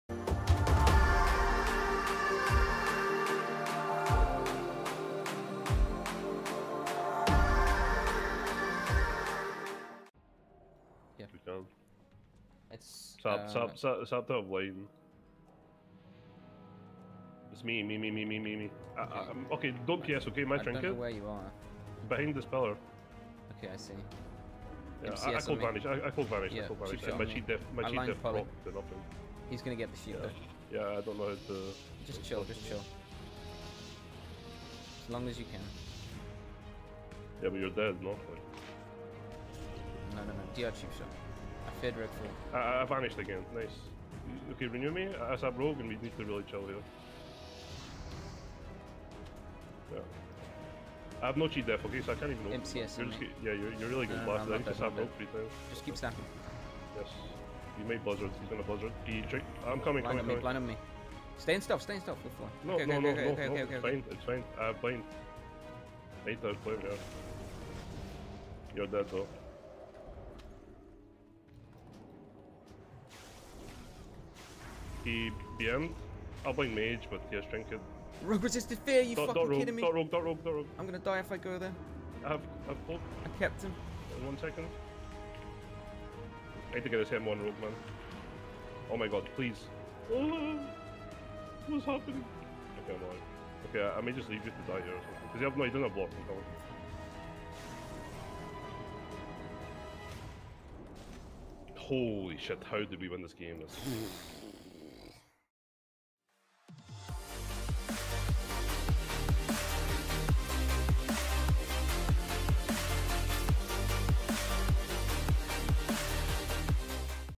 It comes down to a single global
Priest Rogue vs Mage Rogue super close game